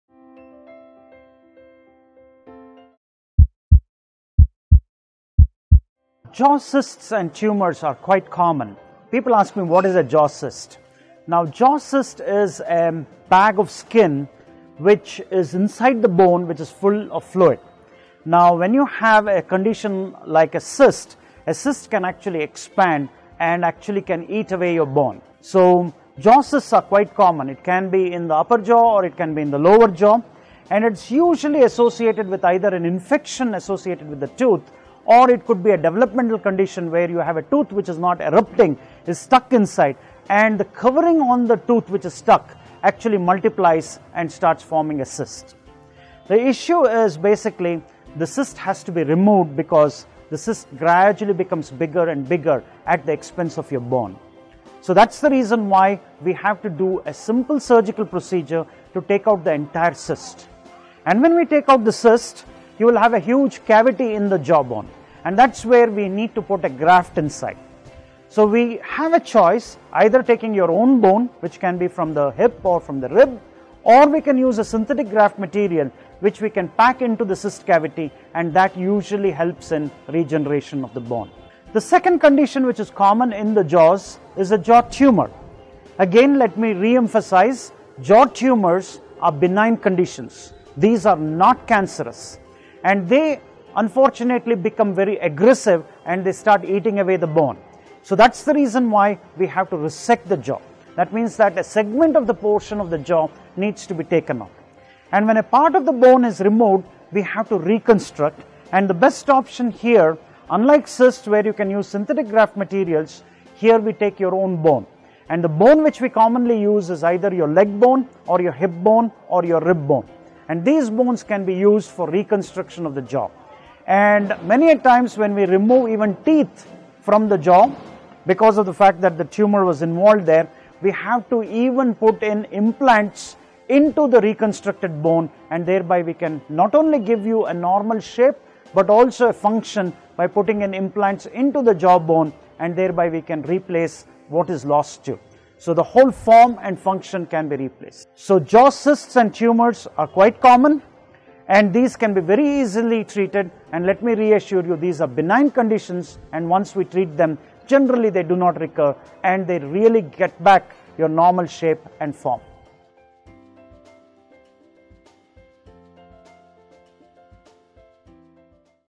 Jaw Cysts & Tumors Diagnosis & Treatment - Dr. Girish Rao
Jaw cyst is a bag of skin which is inside the bone which is full of  fluid.  When you  have a condition  like a cyst, a  cyst  can  actually  expand  and  eat away  the  bone. It  can be in the upper  jaw or the lower  jaw and it is  usually  associated  with an infection associated with the tooth or it could be a developmental  condition where the tooth is not developing  and is stuck  inside and the covering on the  tooth which is  stuck and it  multiplies  and   starts forming a cyst.  The issue is  basically  the cyst has  to  be removed  because the cyst becomes  bigger and  bigger at the  expense of  your  bone. So that is  the reason why you have to do a simple surgical procedure to remove  the entire cyst and when we take  out the cyst we  will have a  huge  cavity in the jaw  bone  and that  is the reason why we have to put  a graft inside. So  we  have a choice, taking out  your  own bone  which can be  form the hip or the rib or  we can use  a synthetic graft material which we can pack  into the  cyst  cavity  and that helps in regeneration of the  bone the  second condition common  in the jaws  is the  jaw tumor. Jaw tumors are benign  conditions  and they are not cancerous  and they unfortunately become very aggressive  as  they  start eating away the  bone and that is the reason why  we have  to resect  the  jaw and that is  why the  segment of the jaw is  taken  out  and  when part of the  jaw is take out, it has to be reconstructed and  the  best  option here  is  unlike cyst where we can use  synthetic graft material, here we take  your  own  bone and the  bone  which we commonly use  is either your leg bone  or  hip bone  or your  rib  bone. These  bones can  be  used for  reconstruction of the  jaw  and  many at times  when we remove teeth from the jaw, because of the fact the  tumor was  involved  there we have  to even  put an implant  into  the reconstructed  bone  and thereby we  can  give  you not only give you a normal shape but  also regain the function and thereby replace what is lost here. So the whole form and function can be replaced. So jaw cysts and  tumors are quite  common and these are benign  conditions and once  we treat them, they do not  reoccur  and once  we treat them. They get back to the normal form.